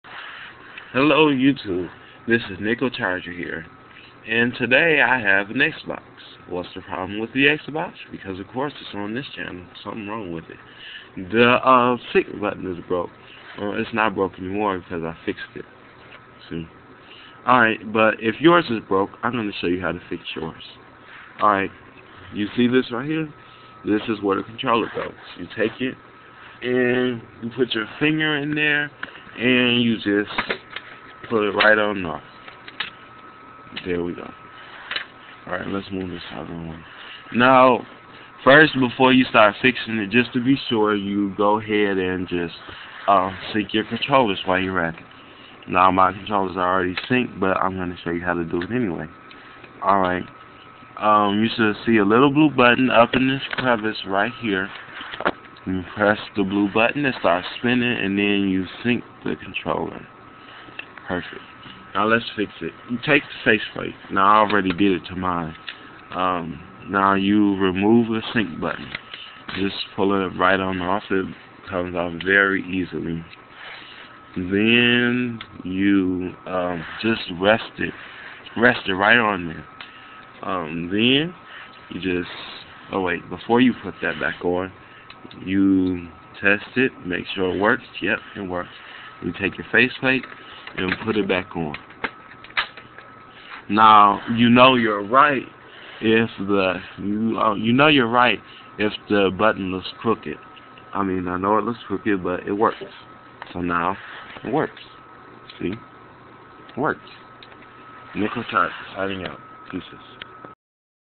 What to do if your xbox 360 will not sync
This is a very simple fix for if you can not sync your controllers.